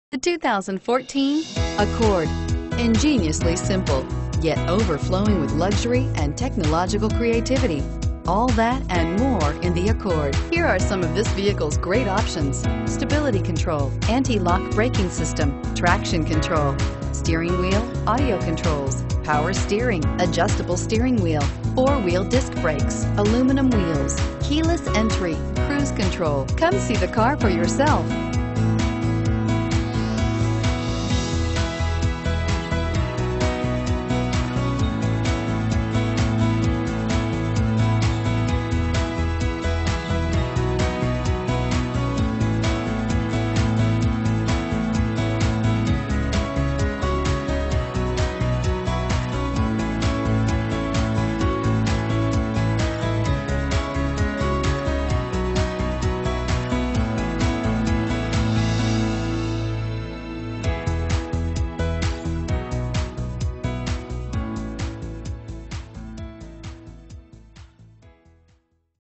2014 Honda Accord Cpe Buford GA 430031
This 2014 Honda Accord Coupe 2dr 2dr I4 CVT LX-S Coupe features a UNSPECIFIED 4cyl Gasoline engine. It is equipped with a Automatic transmission. The vehicle is CRYSTAL BLK PRL with a IVORY CLOTH SEAT TRI Cloth interior. It is offered with a full factory warranty. -